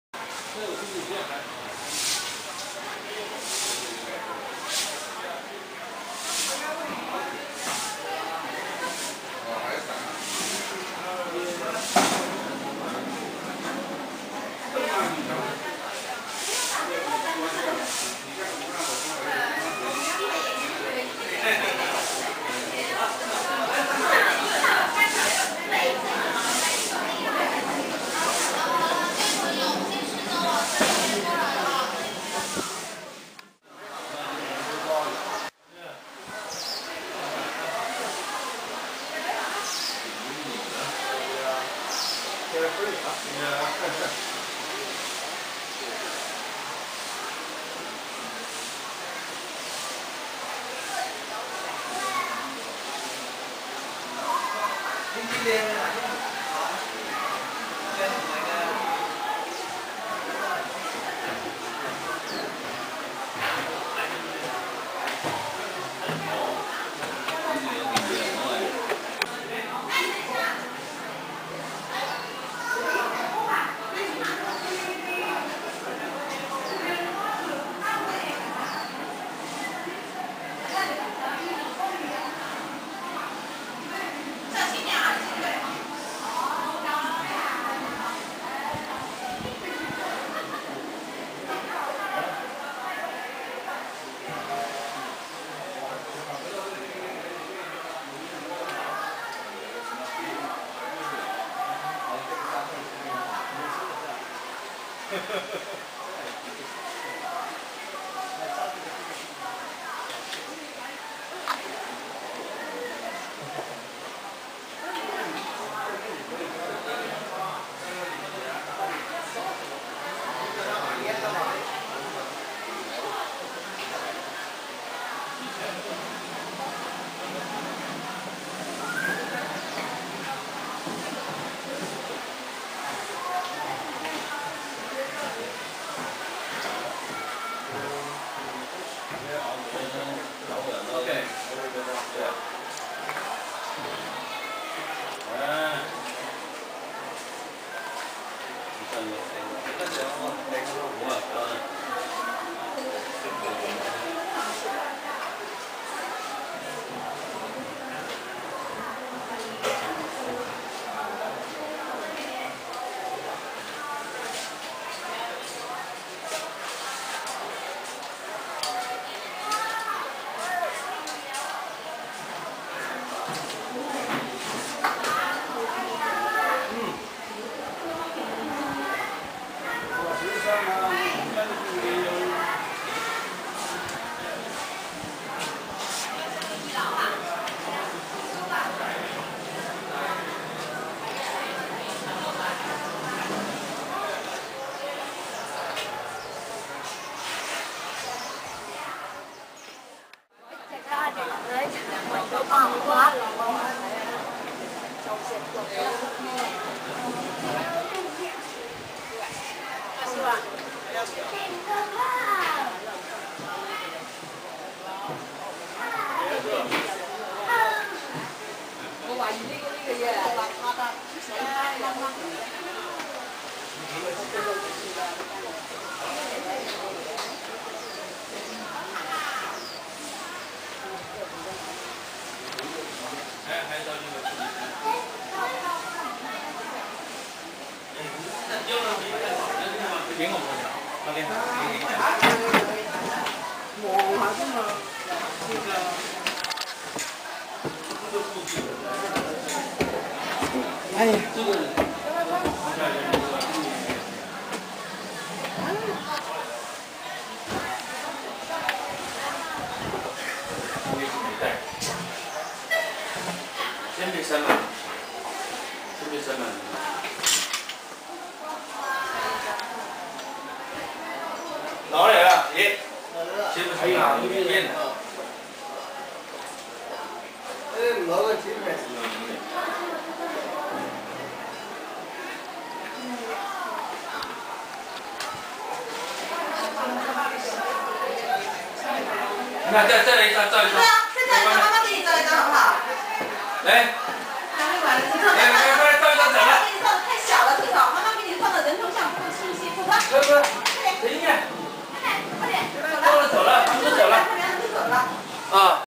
開平_晚香亭內樓上樓下
這段短片保持原聲帶, 讓大家感受一下當時亭樓內的氣氛, 遊人進進出出晚香亭的聲浪暫時覆蓋了四周的風聱、水聲、鳥蟲的音聲 ...